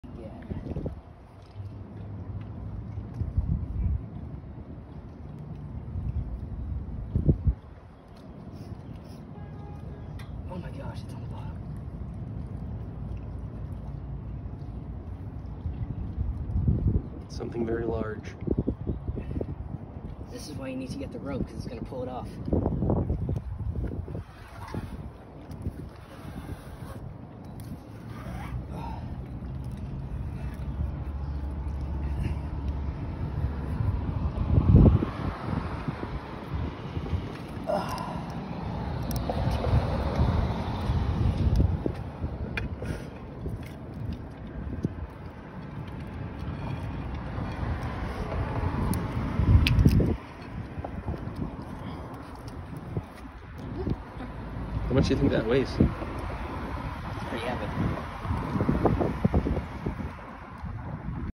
Magnet fishing: car part find
Found a car part in the Kenduskeag. Weighed 8-10 pounds.